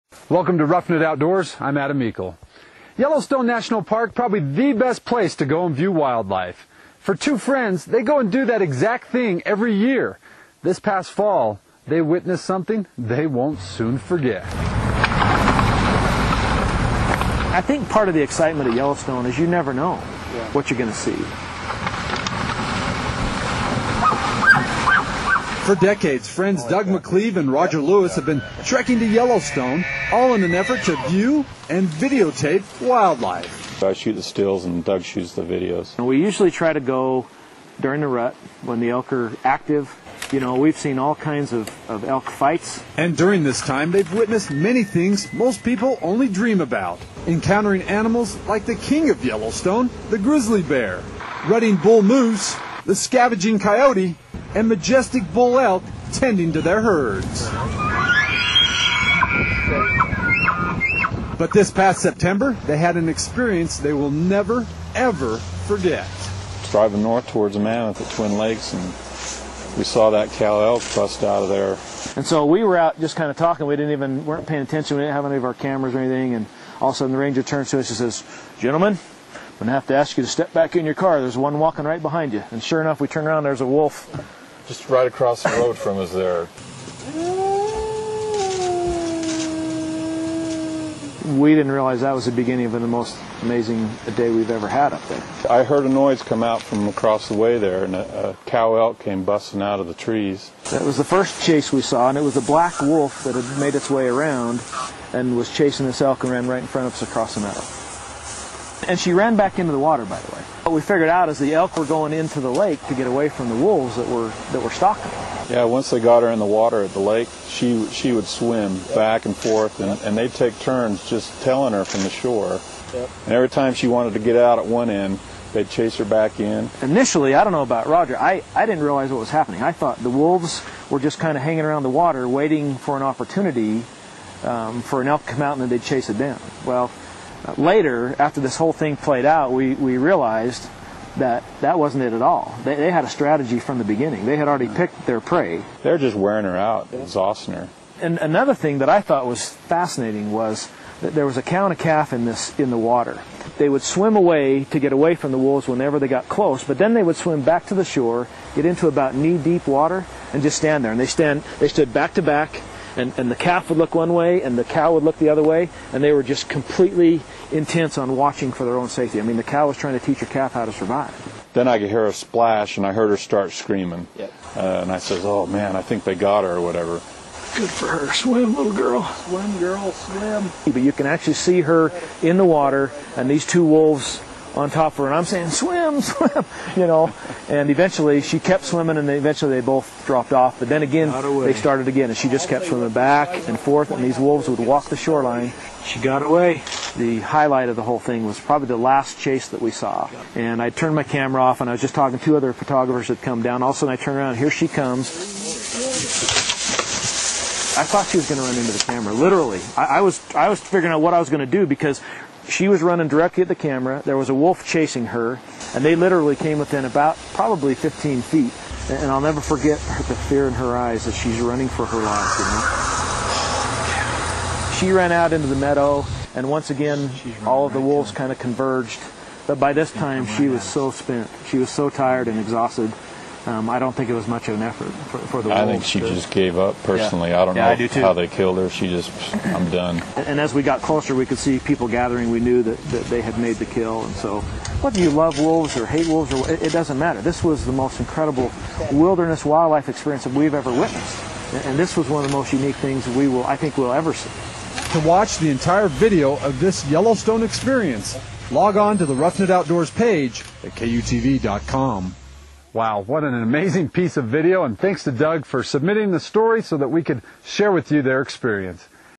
Wolves hunt and kill Elk in Yellowstone
This new Pack hunted and then killed a healthy Cow Elk near twin Lakes September 23, 2008. I watched for 4 hours as they methodically stocked and chased her unyil she basically gave up